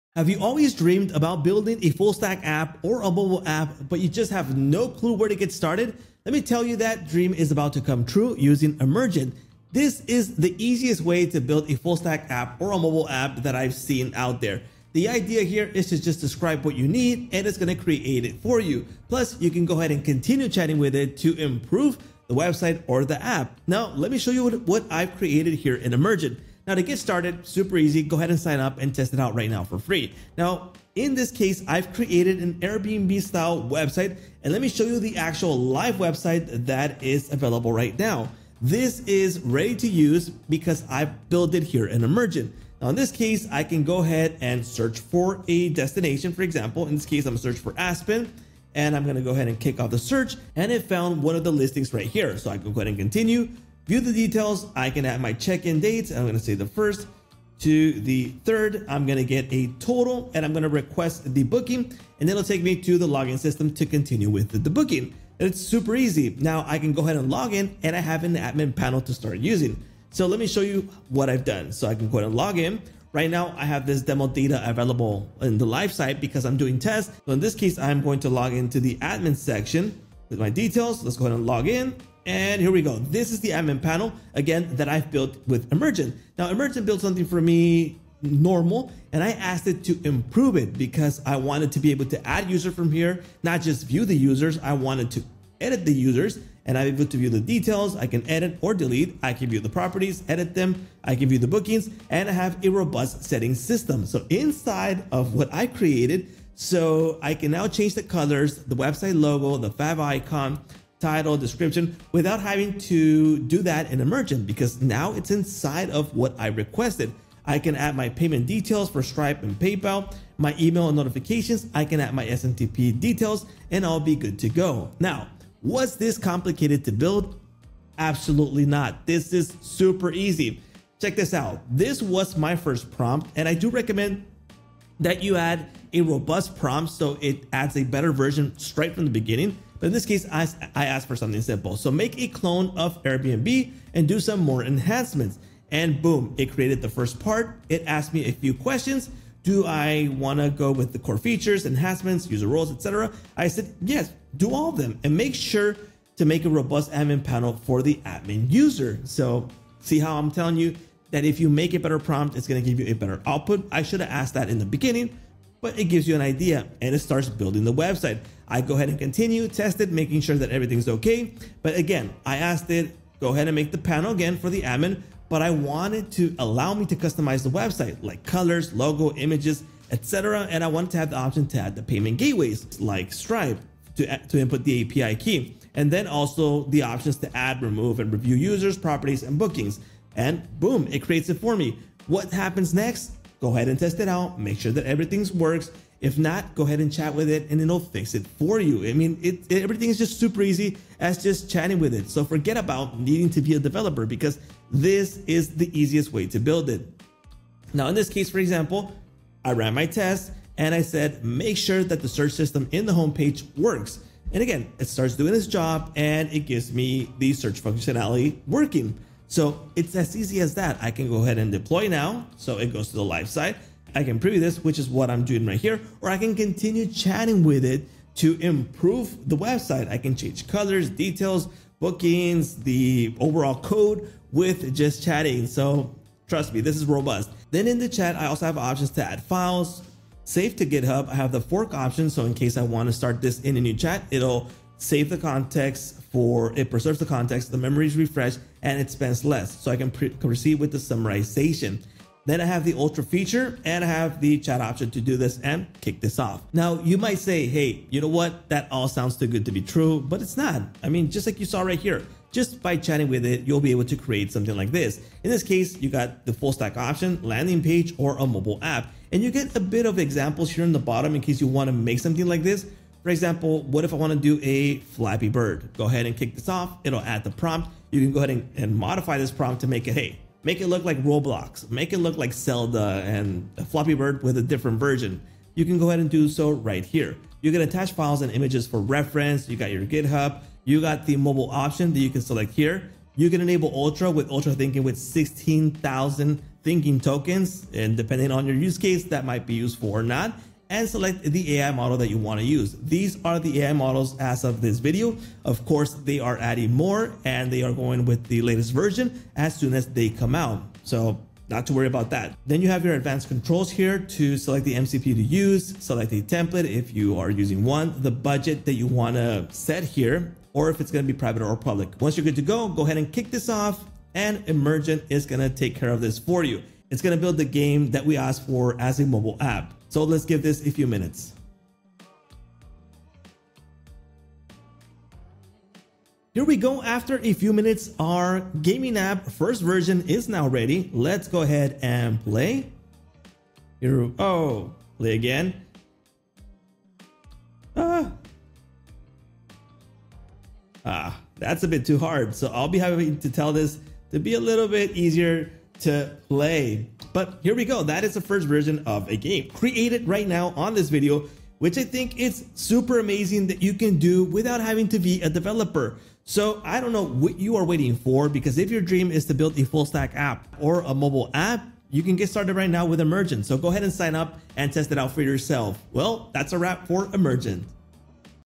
Build Full‑Stack & Mobile Apps with AI (No Code): Emergent Tutorial + Admin, Stripe, Deploy
Want to build a full‑stack web app or a mobile app without being a developer? In this step‑by‑step Emergent tutorial, I show how to go from a single prompt to a live, production‑ready app—complete with an admin panel, user management, Stripe/PayPal payments, SMTP email, and real deployment. I also demo building a quick mobile “Flappy Bird”-style game to show how fast iteration can be with AI.

What you’ll learn:
- How to describe your app and let Emergent generate it (no code required)
- Improving your app by chatting: search, bookings, colors, logos, and more
- Building an admin panel to manage users, properties, bookings, and settings
- Saving to GitHub, forking, using “Ultra” thinking tokens, and choosing AI models
- Deploying to a live site and iterating safely

Perfect for: founders, makers, no‑coders/low‑coders, product managers, designers, and developers who want to move fast.

Chapters
00:00 Intro — Build full‑stack or mobile apps with AI (no code)
00:35 Live demo: Airbnb‑style site generated with Emergent
01:05 Search, listing details, dates, and booking flow
01:30 Auth & Admin: logging in and using the dashboard
01:55 Managing users, properties, bookings (CRUD)
02:15 Site settings: branding, colors, logo, favicon, metadata
02:35 Payments & email: Stripe/PayPal + SMTP configuration
03:00 Getting better results with stronger prompts
03:25 Iterating via chat: fixing search and improving features
03:55 Deploying, previewing live, and continuing to edit
04:20 Advanced tools: files, GitHub save, fork, summarization
04:45 Ultra thinking tokens & choosing AI models
05:10 Mobile app mode: Flappy Bird‑style game generation
05:40 Tuning difficulty and rapid iteration
06:05 When to use budgets, templates, private/public projects
06:30 Final thoughts & how to get started with Emergent

PRO TIPS
- Start with a structured prompt: Define core features, user roles, data models (Users, Properties, Bookings), admin requirements, payment providers, and customization options (colors/logo) in your very first prompt. You’ll get a stronger initial build and fewer fixes later.
Run end‑to‑end test bookings and emails before switching to live keys to avoid failed payments or missing notifications.
- Version control your iterations: Save to GitHub, use forks for bigger changes, and summarize context before long sessions. This keeps your app stable, makes rollbacks easy, and reduces token spend when projects get large.